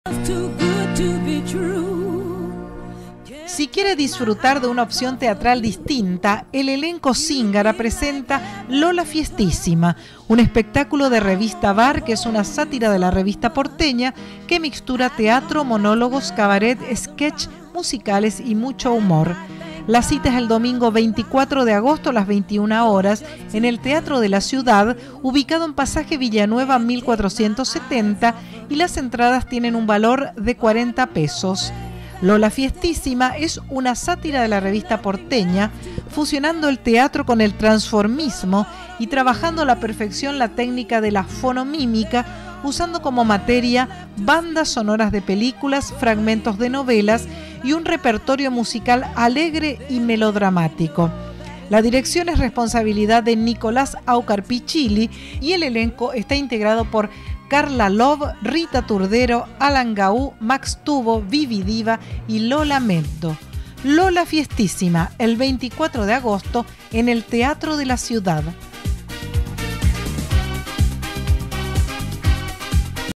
LOLA Fiestísima por Zingara
Espectáculo a presentarse el 24 de Agosto en el Teatro de la Ciudad . . . .